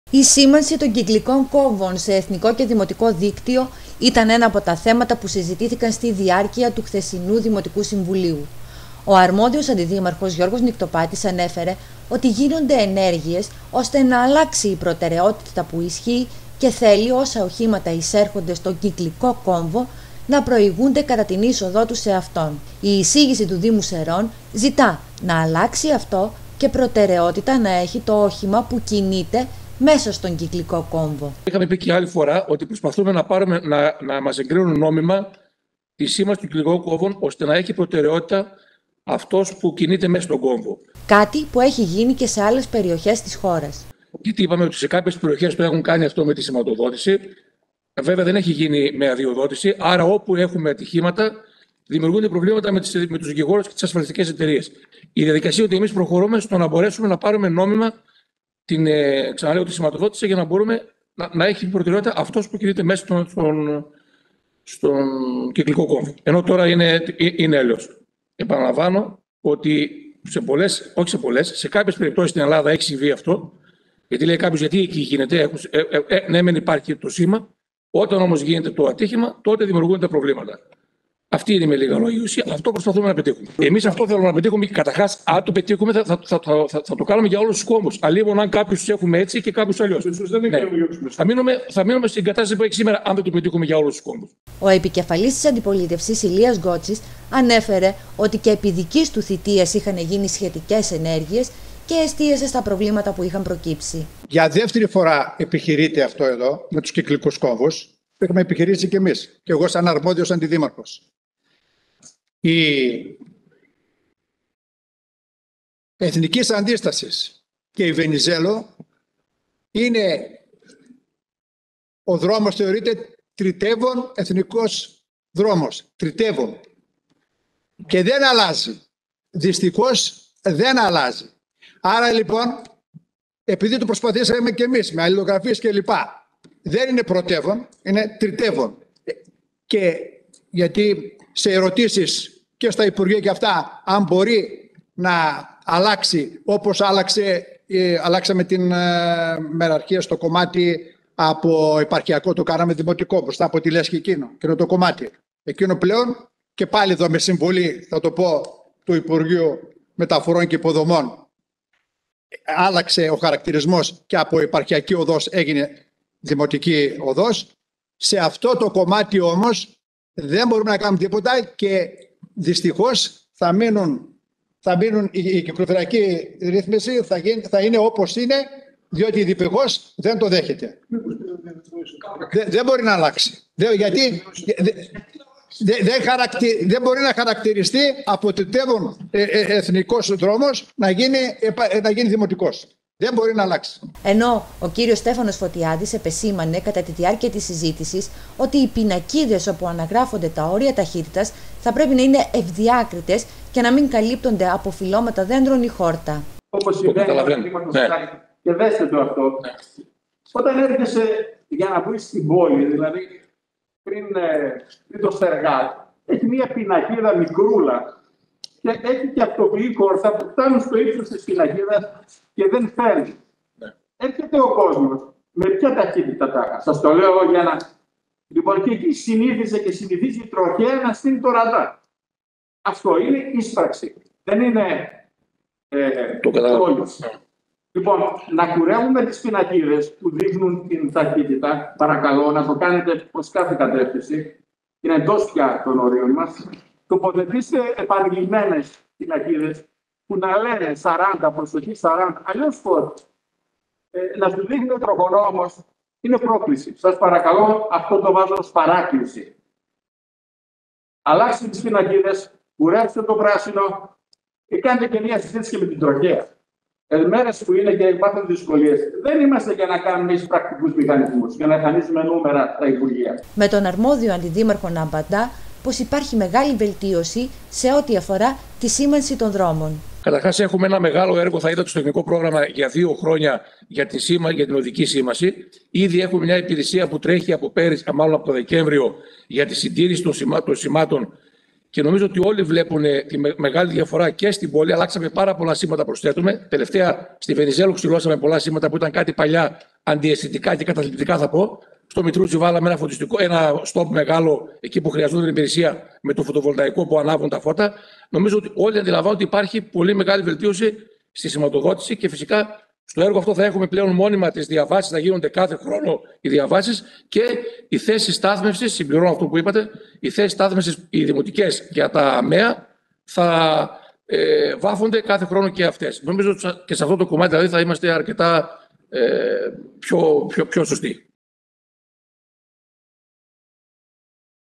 Κυκλικοί Κόμβοι: Την αλλαγή προτεραιότητας εισηγείται η δημοτική αρχή -video
Η αλλαγή προτεραιότητας των κυκλικών κόμβων ήταν ένα από τα θέματα που συζητήθηκαν στη διάρκεια του χθεσινού δημοτικού συμβουλίου. Από πλευράς της δημοτικής αρχής γίνονται ενέργειες ώστε να αλλάξει η ισχύουσα προτεραιότητα, ενώ και οι επικεφαλής των δημοτικών παρατάξεων έκαναν τις δικές του επισημάνσεις.